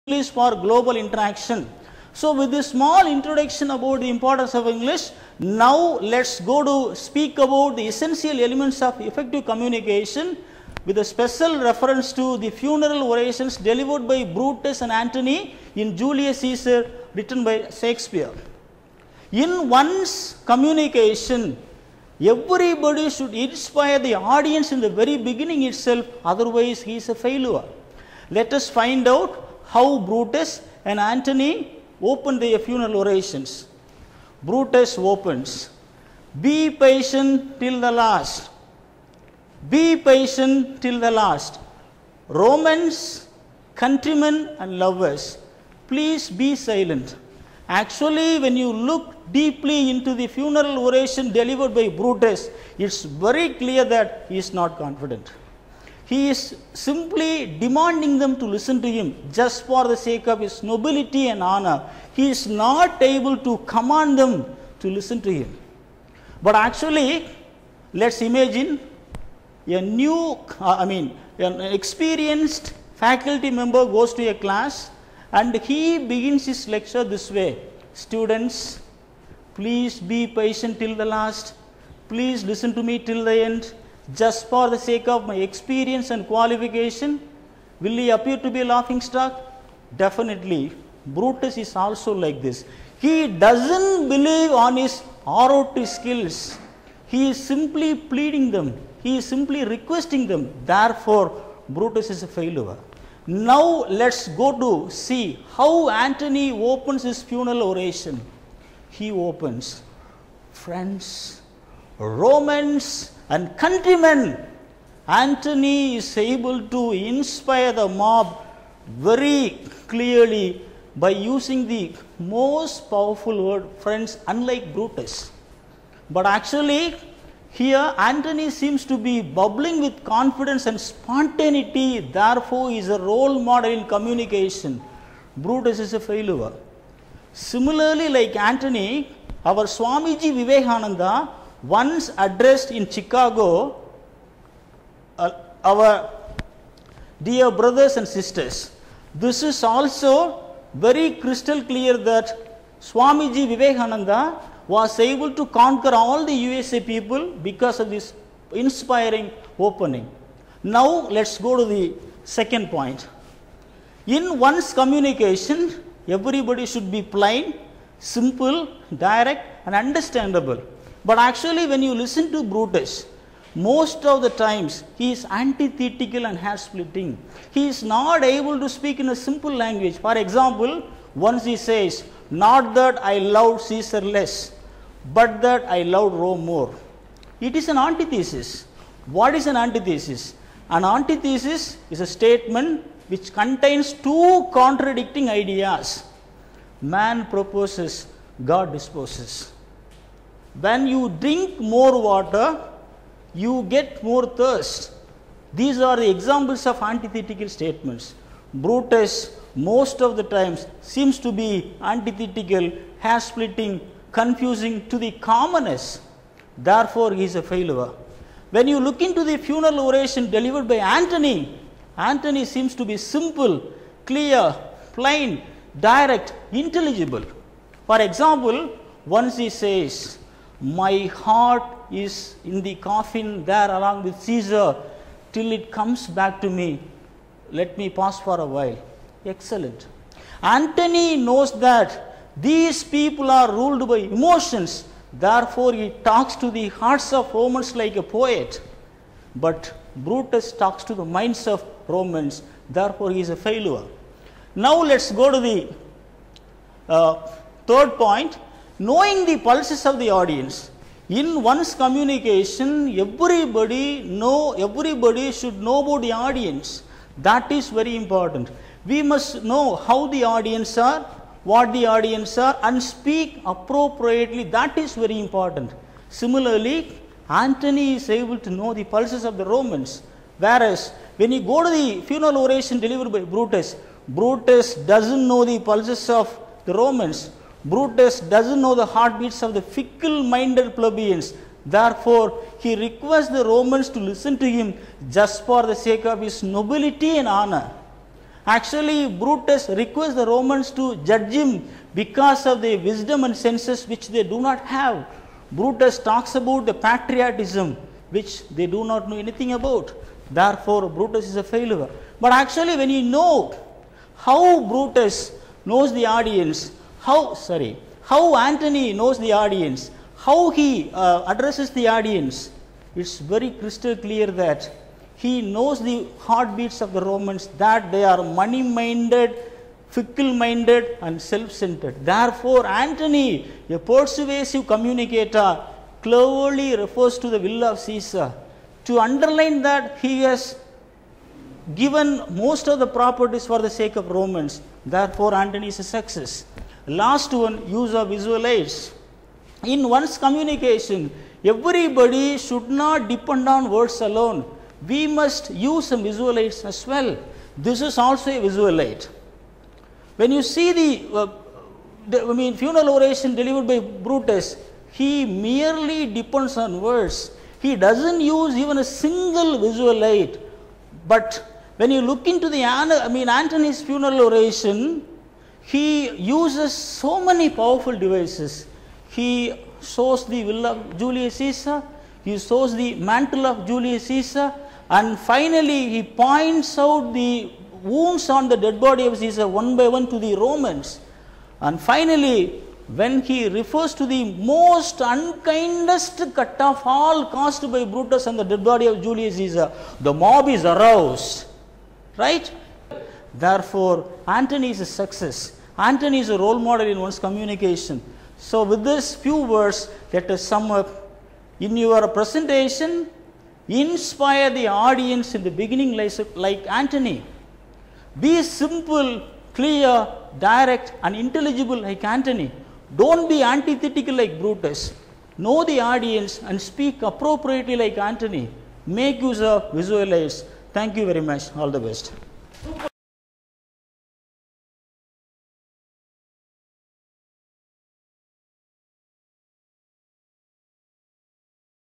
Elements of Effective Communication
Dr.Solaimalai RAJARAM, Professor of English, Thiagarajar College of Engineering, Madurai, Tamil Nadu, India . Attempt a Listening Test on Elements of Effective Communication @